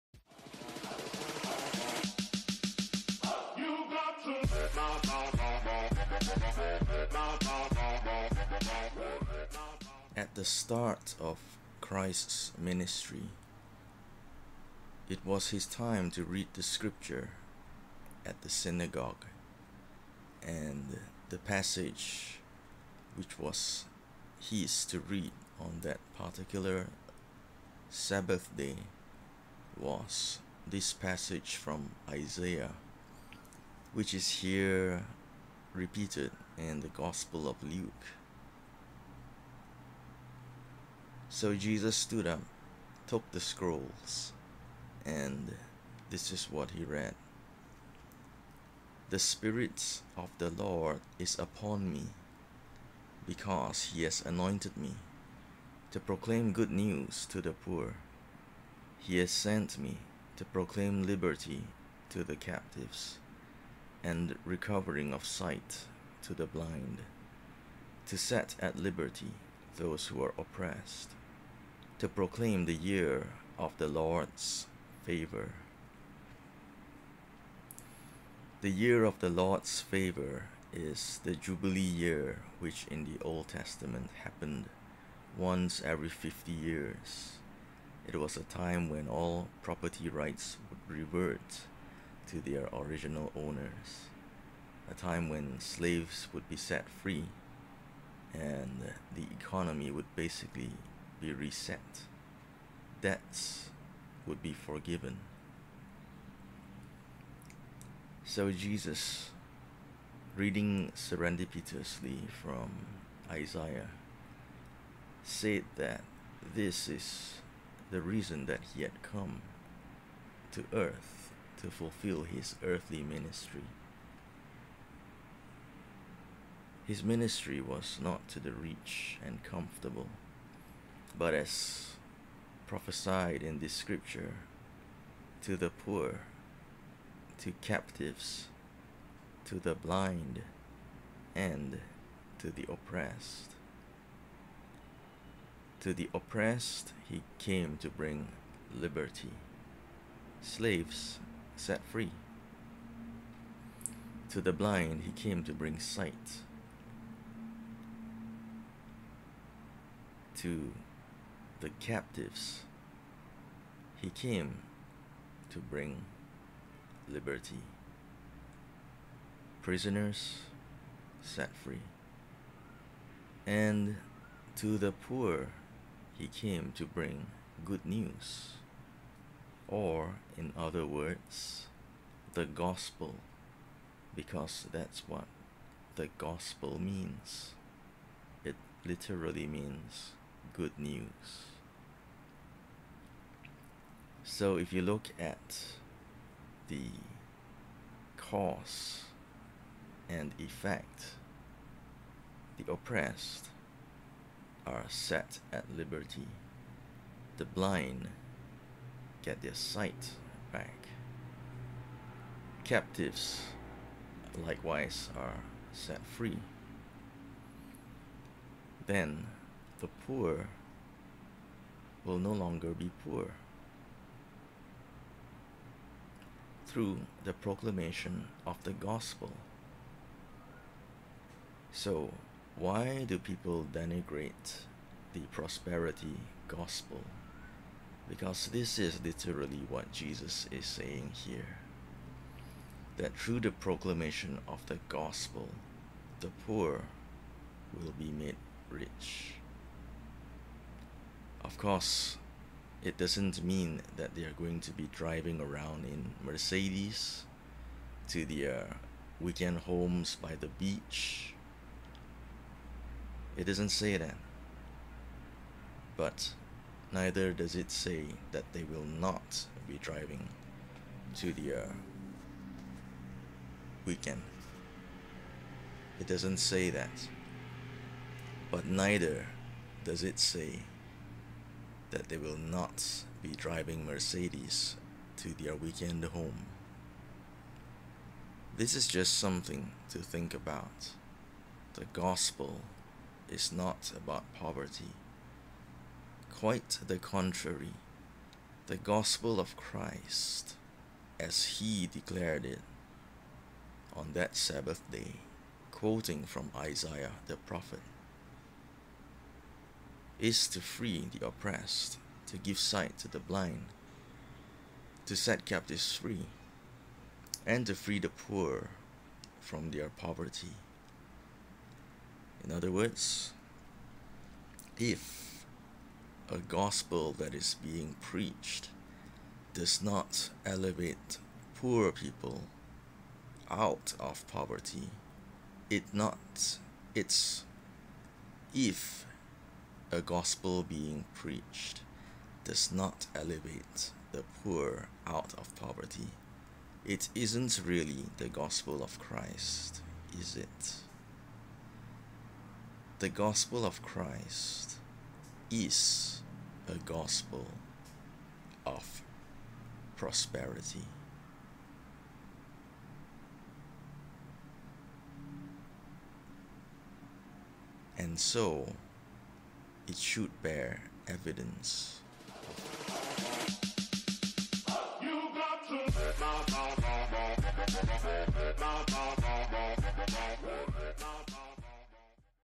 Last Taboo of the Gospel of Christ: Prosperity
What did Jesus say about his own mission here on earth? What, according to him, are the evidences that the Gospel is being preached?

Alpha Lim
Mystic Preacher Man
Real Life Work From Home Daddy

👆 Love Liberty Prosperity